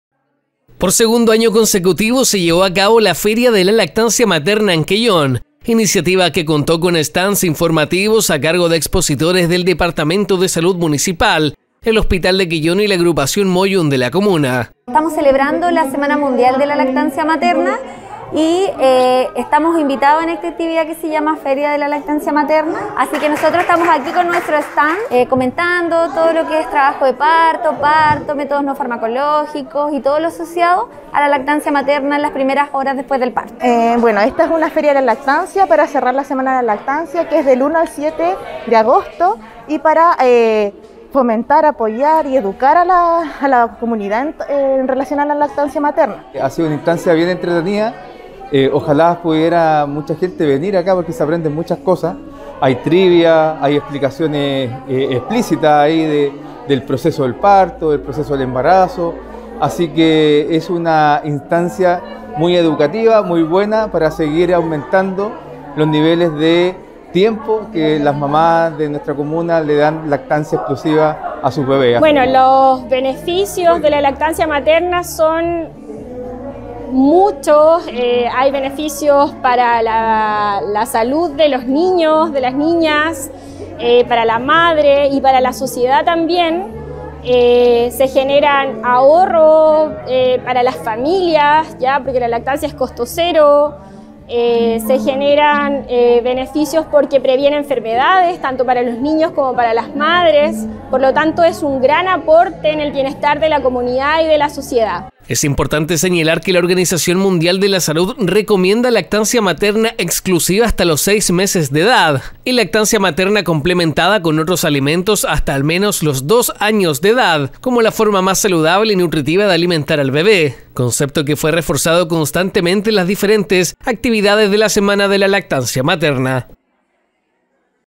Realizan Segunda Feria de la Lactancia Materna en Quellón | Quellón Informa
▶️ Debido a la “Semana Mundial de la Lactancia Materna”, se desarrollaron durante la semana una serie de actividades en la comuna. Una de ellas, fue la segunda edición de la Feria de la Lactancia Materna en Quellón, iniciativa que contó con stands informativos a cargos expositores del Departamento de Salud Municipal, el Hospital de Quellón y la agrupación Moyün de la comuna, con el objetivo de entregar información técnica, y sobre todo fomentar la lactancia materna.